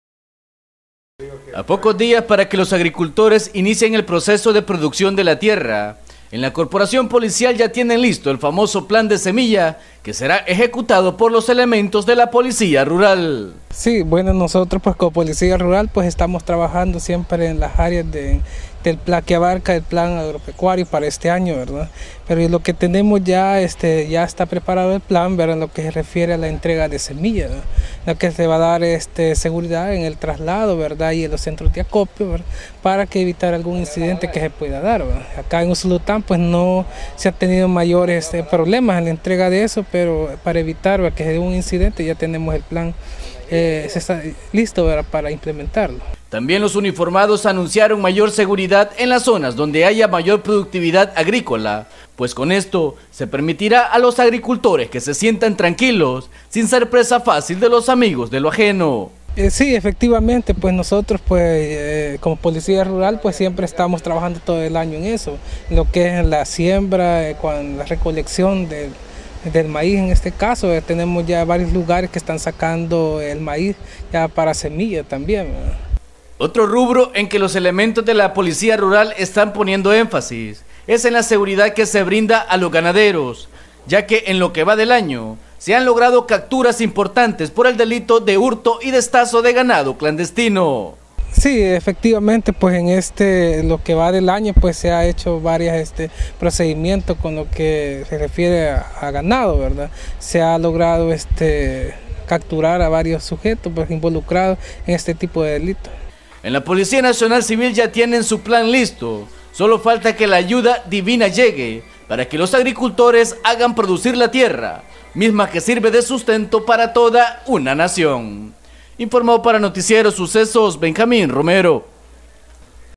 Plan seguridad semilla rural
Ya esta listo el plan de semillla que ejecuta la policía nacional civil, con esto se estara brindando seguridad en todas las zonas productoras de la canasta básica